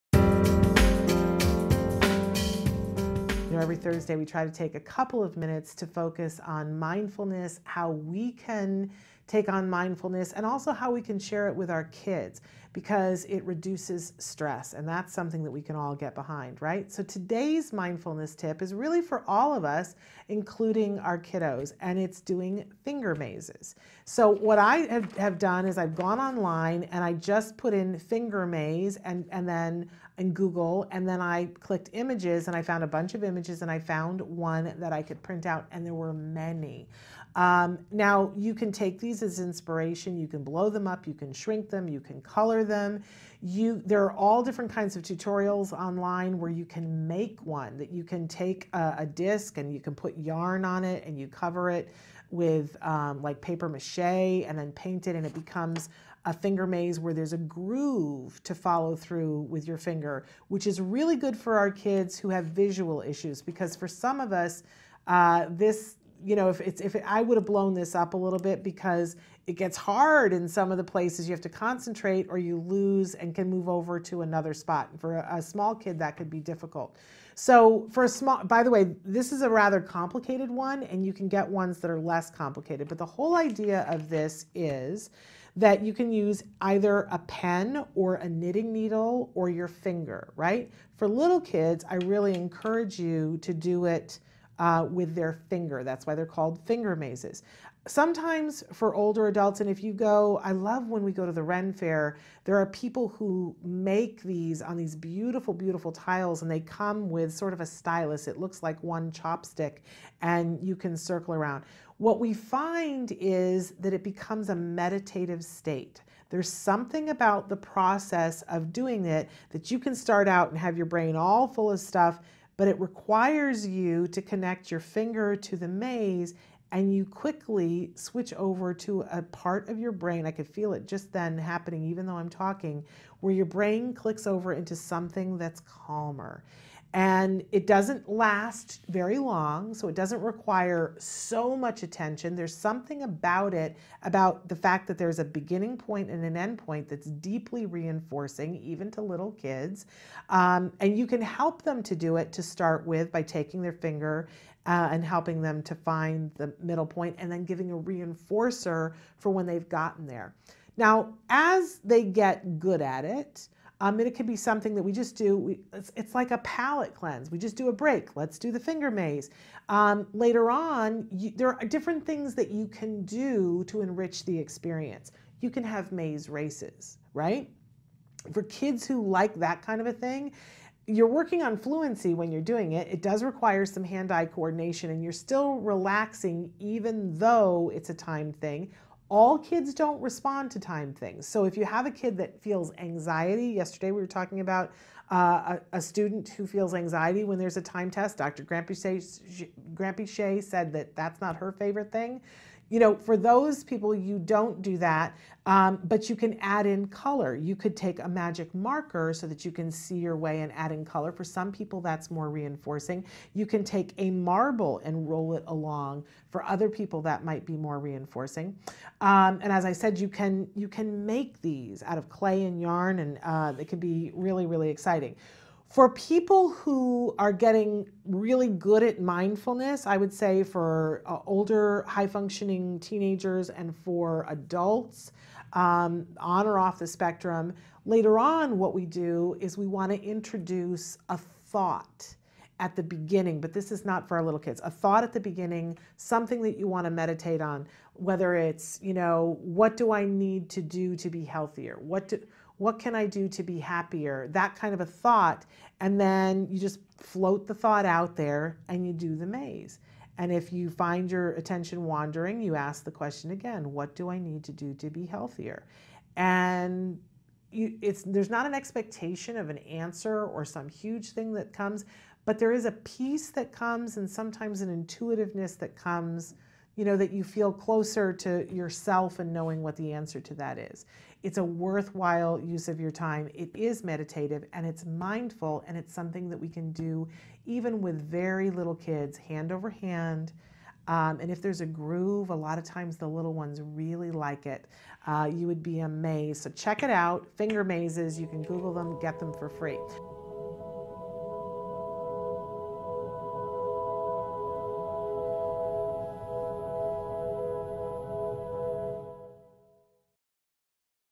Mindful Thursday- Finger Mazes
Shannon offers her weekly Mindful Moment for parents and children to encourage relaxation, focus, and being present in the moment.